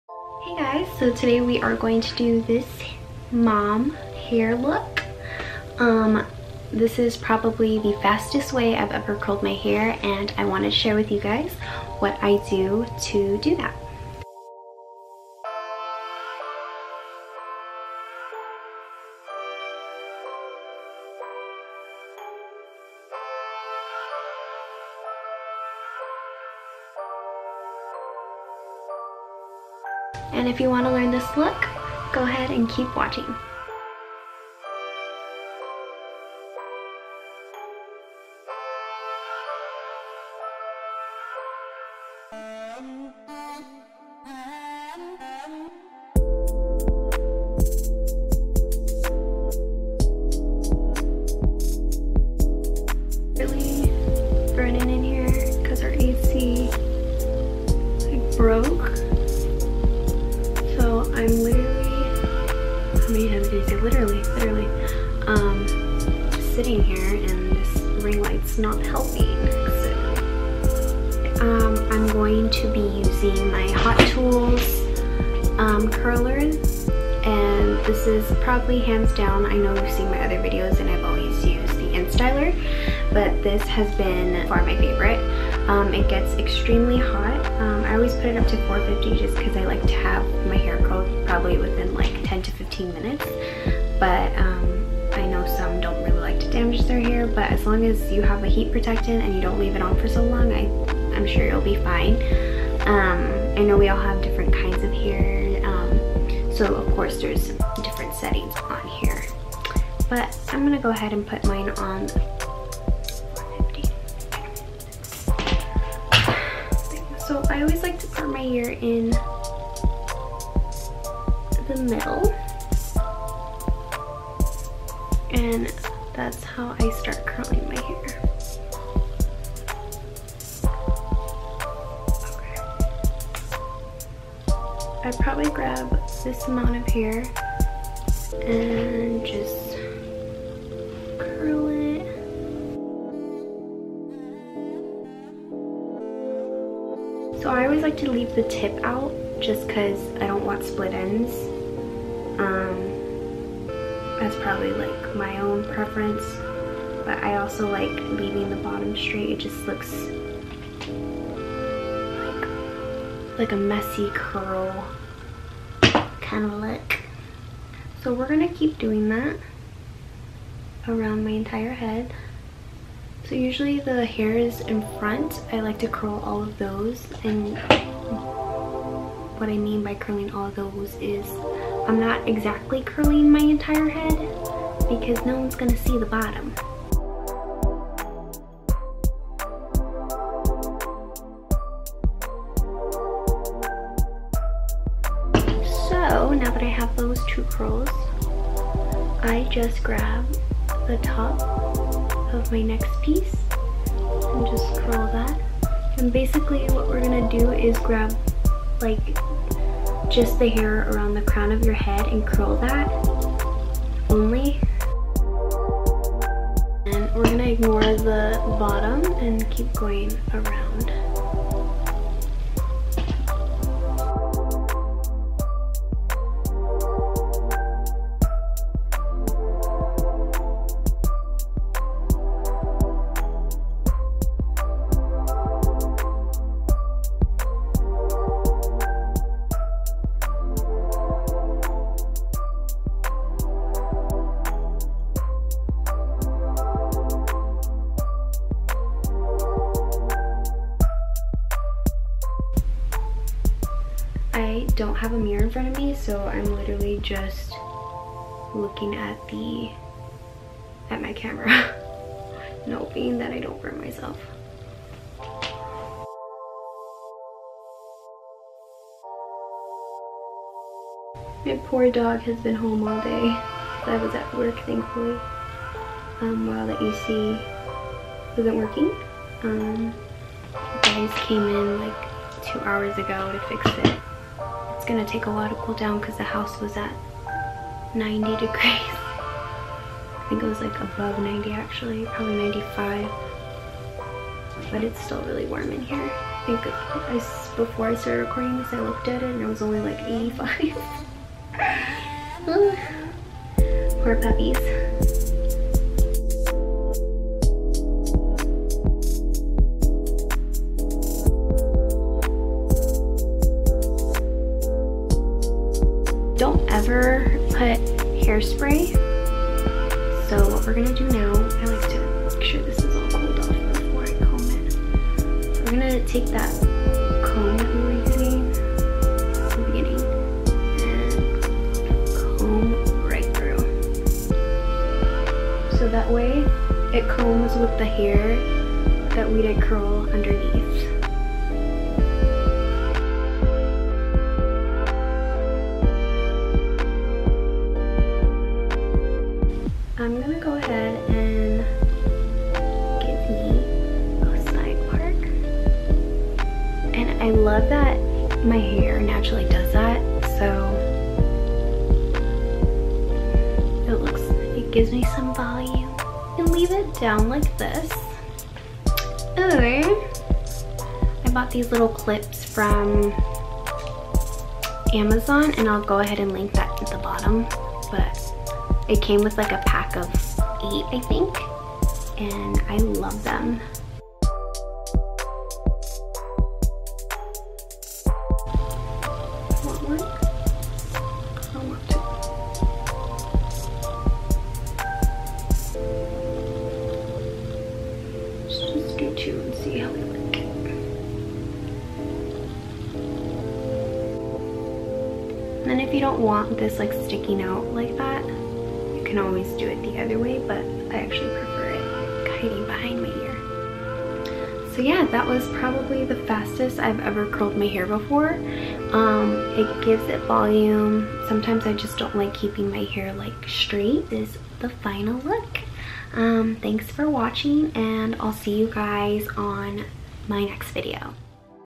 MOM HAIR LOOK
Hello everyone!

Heres a small, quick, and easy tutorial on how to curl your hair in 10 mins and be on your way!

Products used:
-OGX Protecting + Silk Blowout Quick Drying Thermal Spray

-Wide tooth comb

-Hot Tools Curling Iron/Wand 1inch (mines is in pink but heres one on sale)

-Acrylic hair barrettes